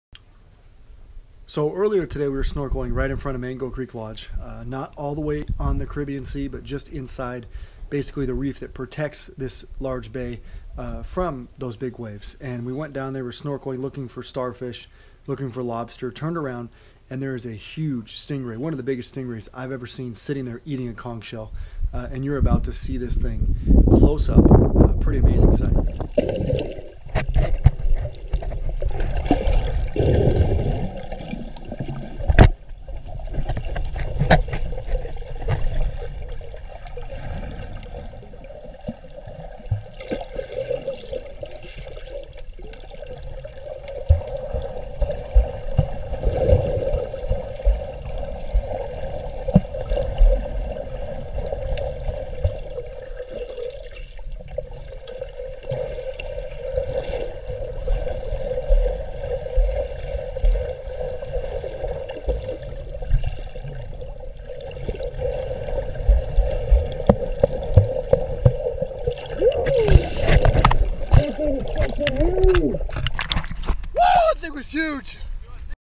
Roatan Huge Sting Ray Encounter
We were snorkeling off the coast of Roatan, Honduras, in front of Lime Cay scowering the bottom in search of critters when we noticed a huge sting ray eating a conch. Rather than look from afar, I dove down for a close encounter and came face to face with this gentle giant.